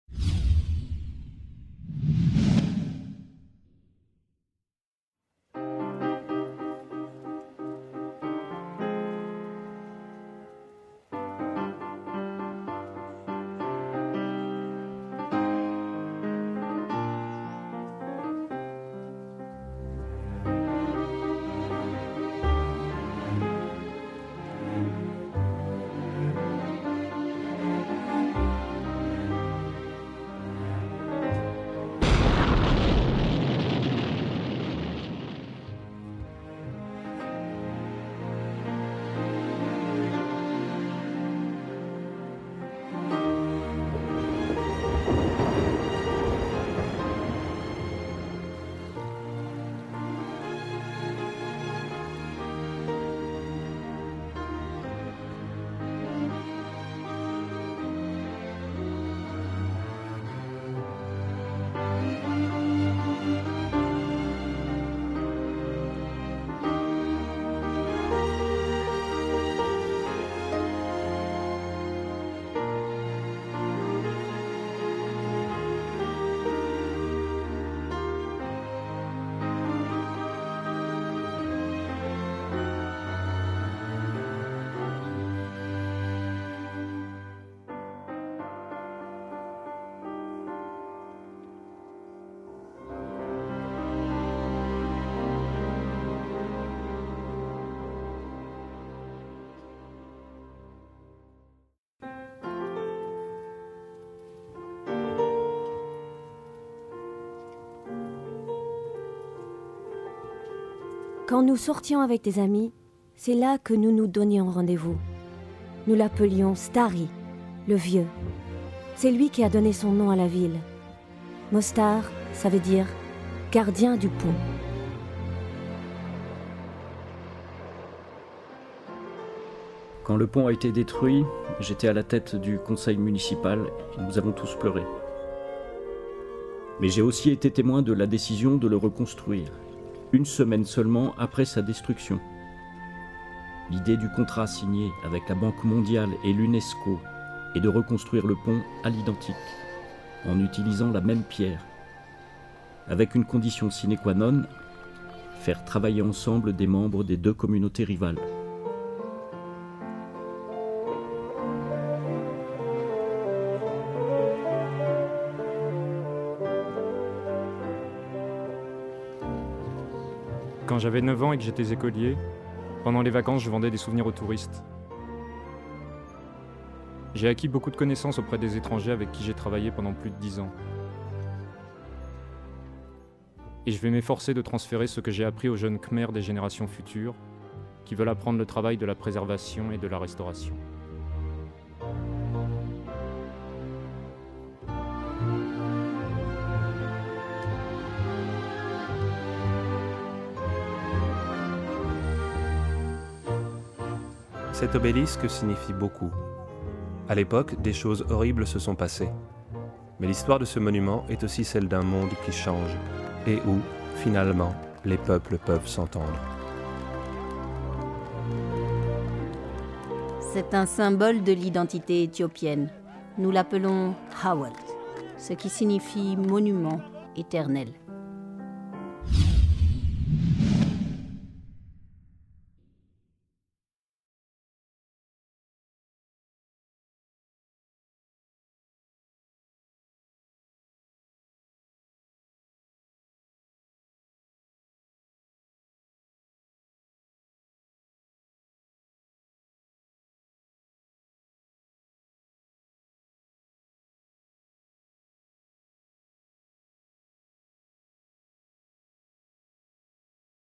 World Heritage - Dangers and Success Stories
Celebrating 40 years of World Heritage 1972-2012

The World Heritage Convention is the most ratified international treaty for cultural and natural heritage preservation in the world. This year, on its 40th anniversary, the World Heritage Convention celebrates sustainable development and the role of local communities.